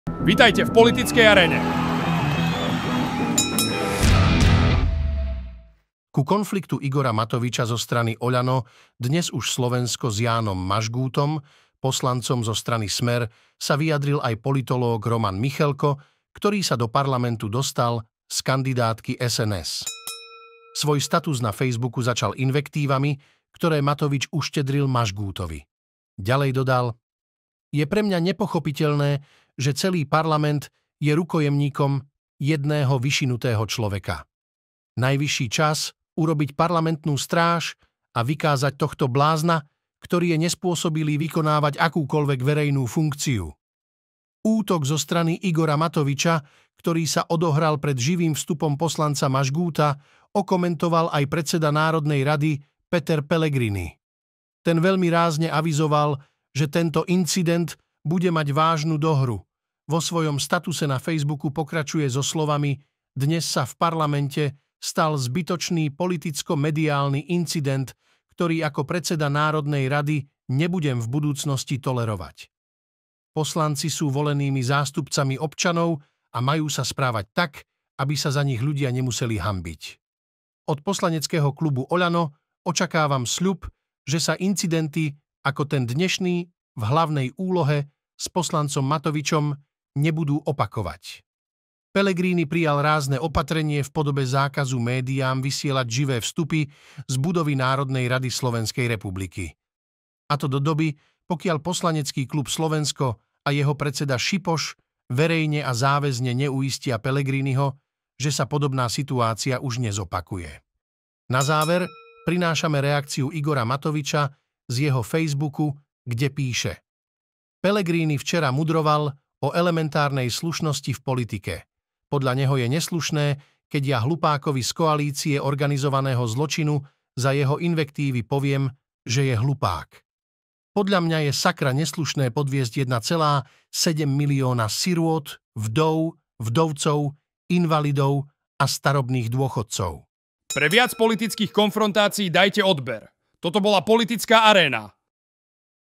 Podľa MICHELKA je vyšinutý blázon, PELLEGRINI ho uzemnil ako malé dieťa.
Prinášame vám reakciu Romana Michelka, Petra Pellegiriniho na vzniknutý slovný konflikt medzi Igorom Matovičom a Jánom Mažgútom. Na záver prinášame vyjadrenie Igora Matoviča.